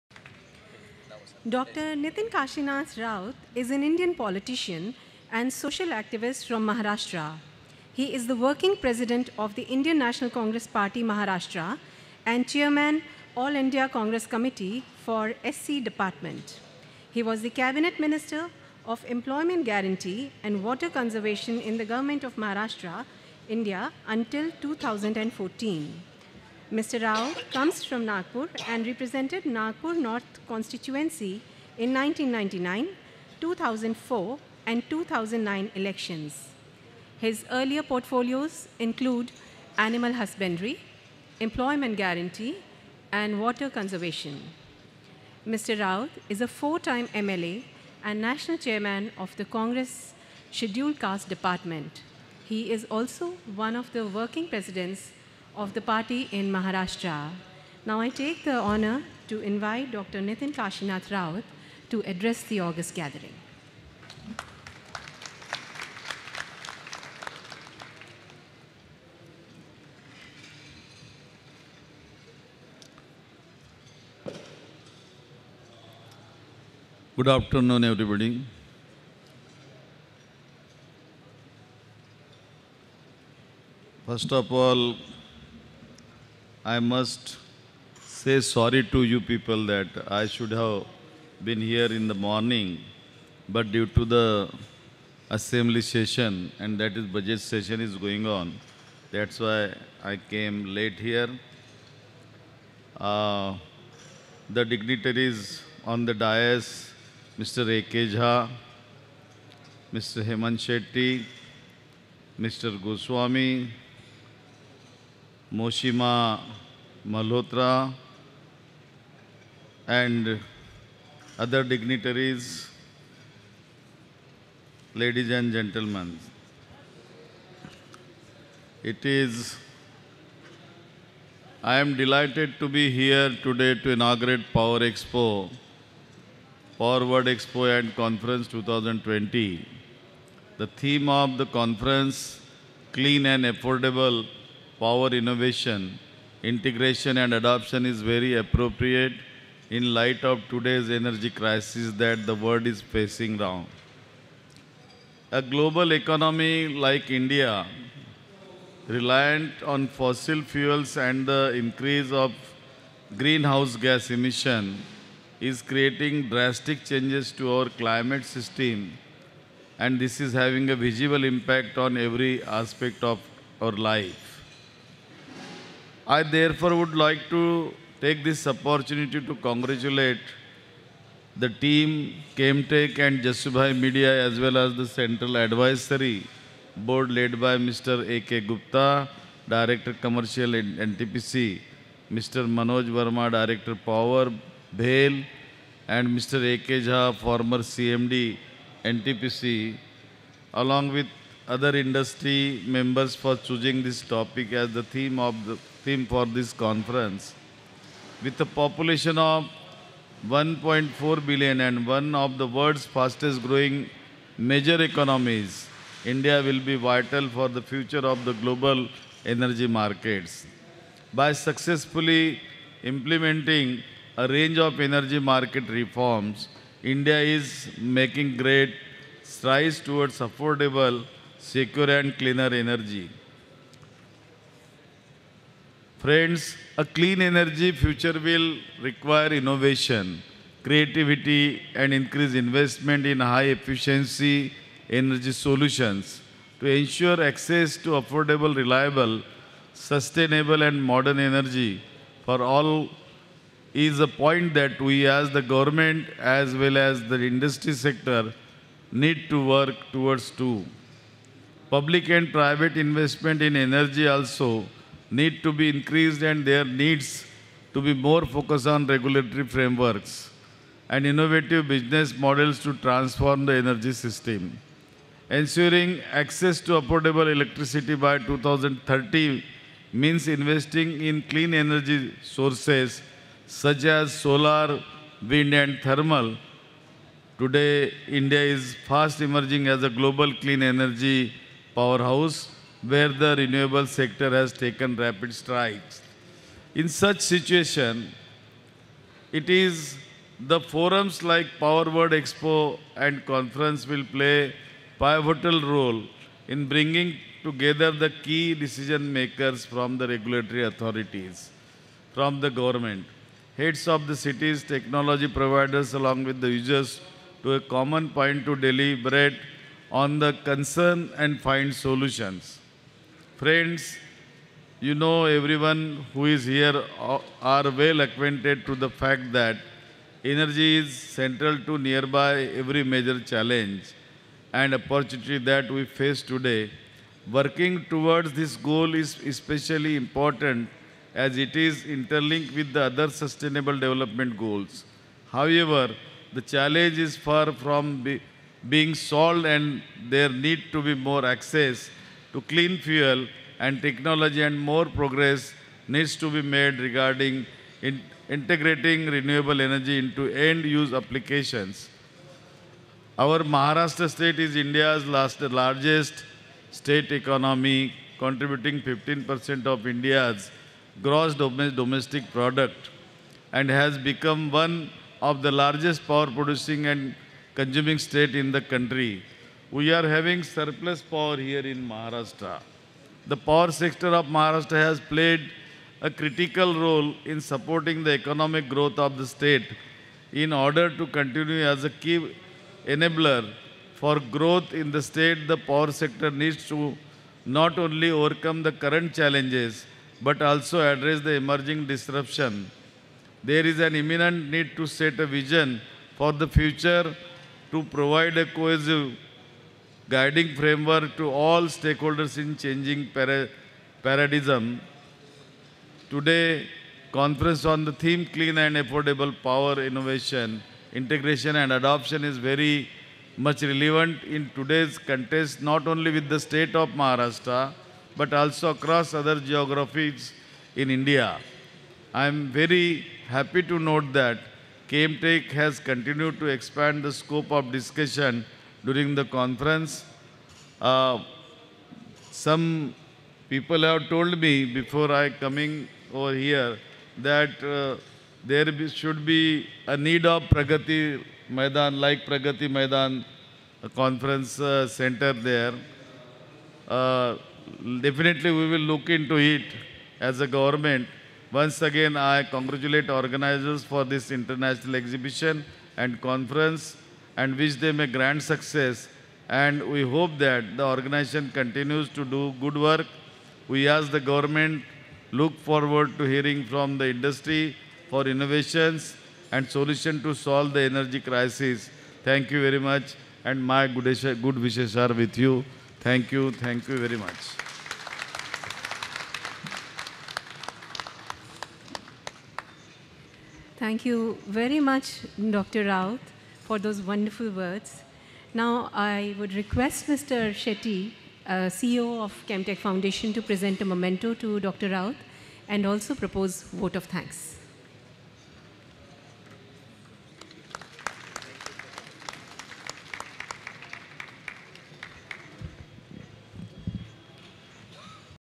"India is vital for being a future of global energy market" Dr. Nitin Kashinath Raut, Govt of india
Dr. Nitin Kashinath Raut, Hon'ble Minister - New and Renewable Energy - Government of Maharashtra, Power World 2020 Conference Program talked about the theme of Clean & Affordable Power: Innovation, Integration & Adoption in the Bombay Exhibition Centre, Goregaon on 5th March 2020.